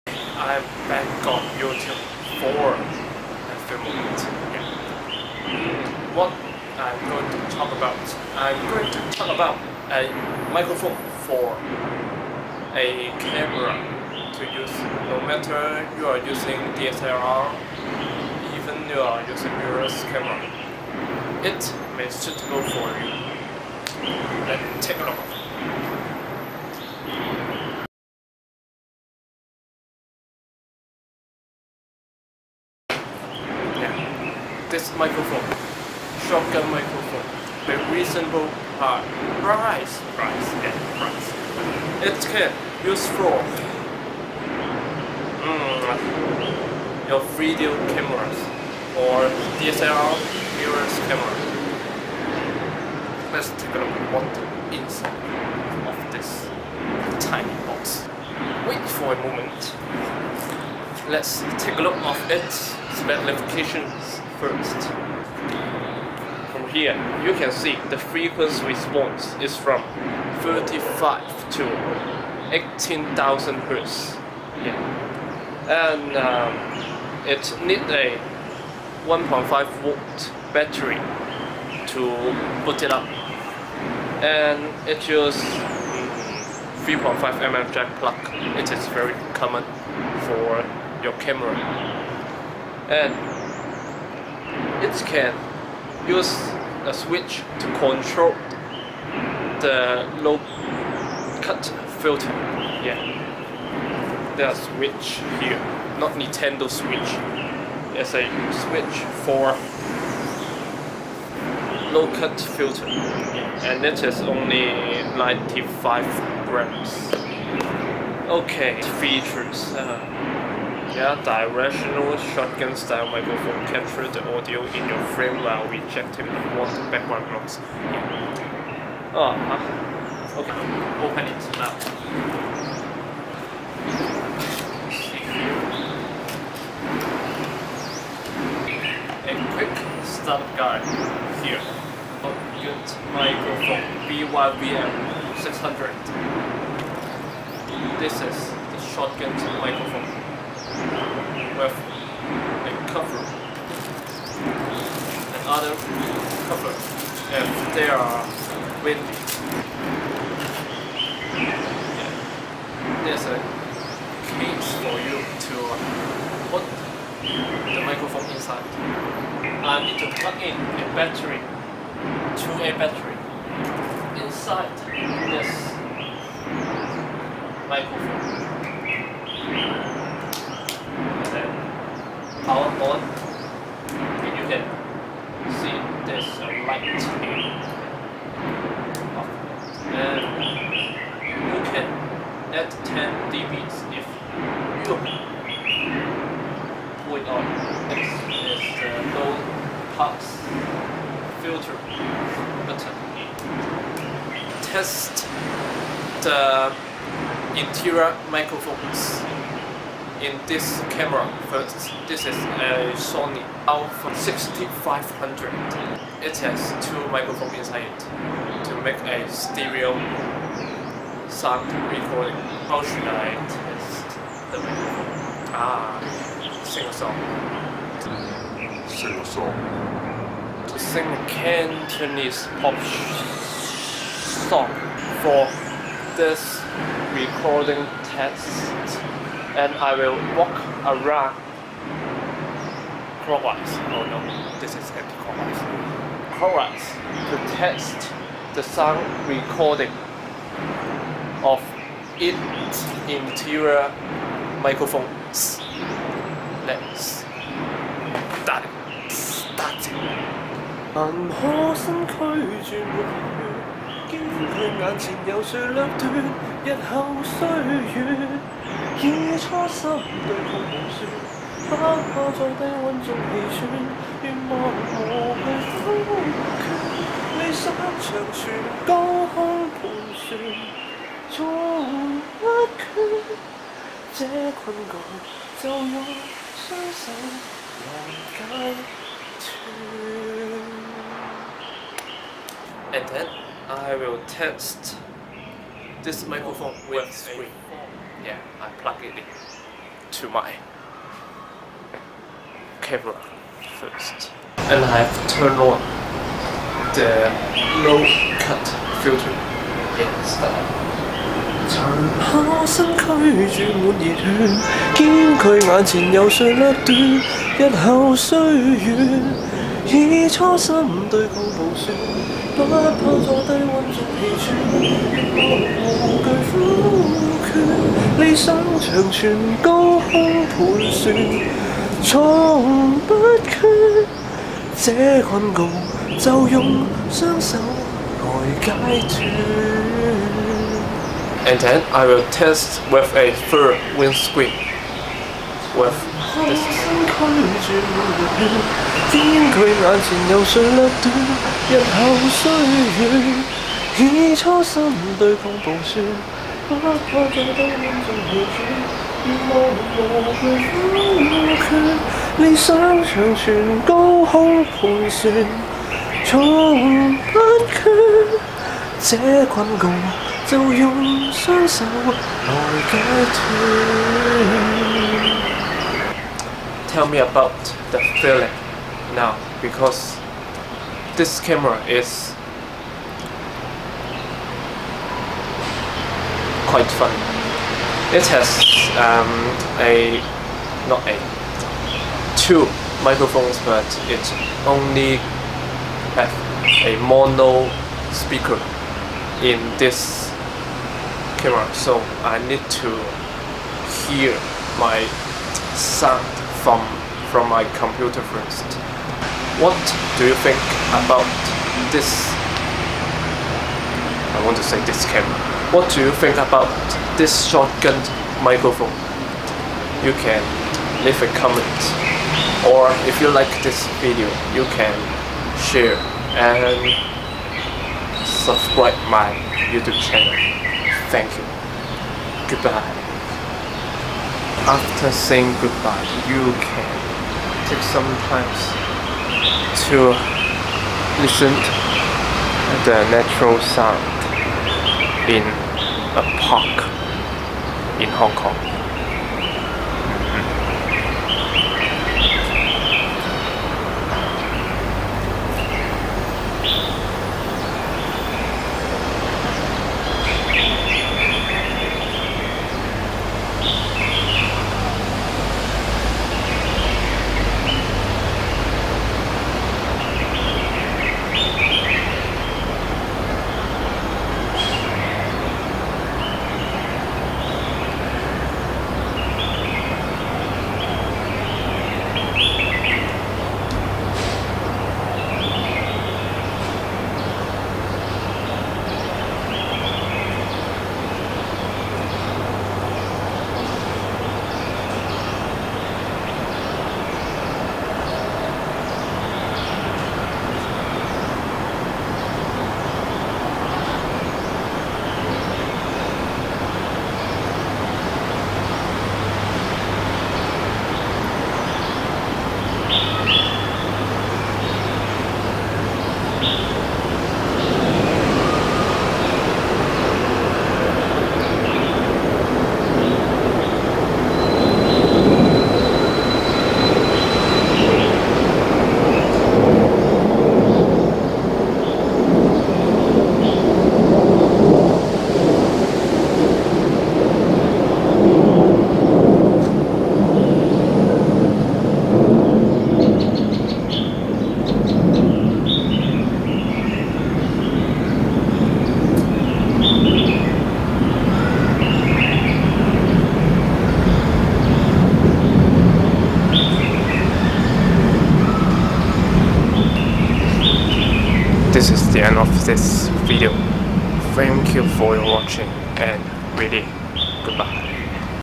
Unboxing BOYA VM600 Shotgun Microphone |English 4K
This video is about the BOYA Shotgun Microphone VM600 and this video include some rough testing, there is also a Cantonese Version of this video and I will upload it soon.